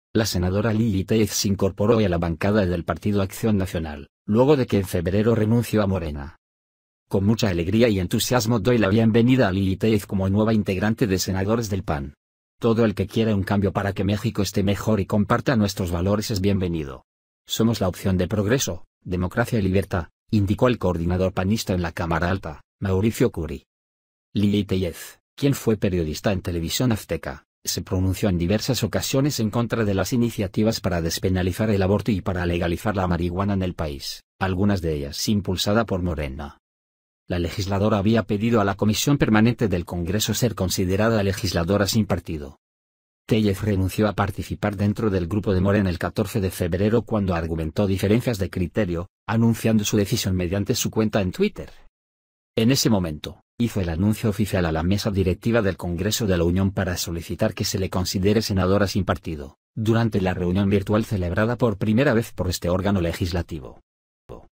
Lilly Téllez se une al PAN tras romper con Morena
La senadora Lilly Téllez se incorporó hoy a la bancada del Partido Acción Nacional, luego de que en febrero renunció a Morena.

“Con mucha alegría y entusiasmo doy la bienvenida a Lilly Téllez como nueva integrante de Senadores del PAN. Todo el que quiera un cambio para que México esté mejor y comparta nuestros valores es bienvenido. Somos la opción de progreso, democracia y libertad”, indicó el coordinador panista en la Cámara Alta, Mauricio Kuri.

Lilly Tellez, quien fue periodista en TV Azteca, se pronunció en diversas ocasiones en contra de las iniciativas para despenalizar el aborto y para legalizar la mariguana en el país, algunas de ellas impulsada por Morena.

La legisladora había pedido a la Comisión Permanente del Congreso ser considerada legisladora sin partido.

Téllez renunció a participar dentro del grupo de Morena el 14 de febrero cuando argumentó “diferencias de criterio”, anunciando su decisión mediante su cuenta en Twitter.

En ese momento, hizo el anuncio oficial a la Mesa Directiva del Congreso de la Unión para solicitar que se le considere senadora sin partido, durante la reunión virtual celebrada por primera vez por este órgano legislativo.